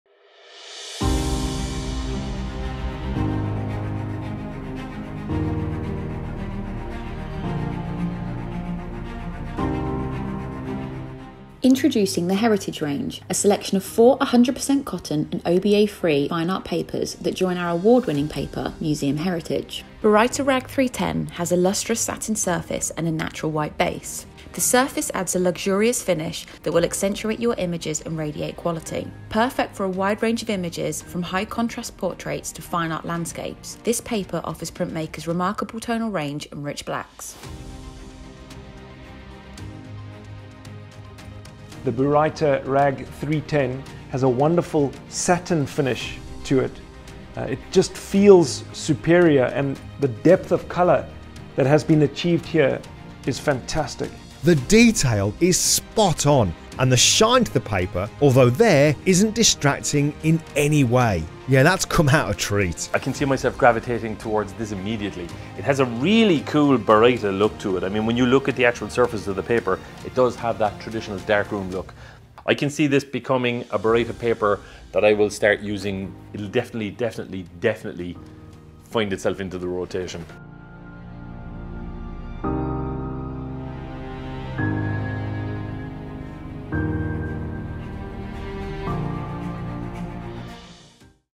PermaJet Heritage Baryta Rag 310 Overview | Satin Fine Art Inkjet Paper
Baryta Rag 310 is a new fine art paper from PermaJet's Heritage Range. Discover the qualities of this paper and see how it prints in this overview video.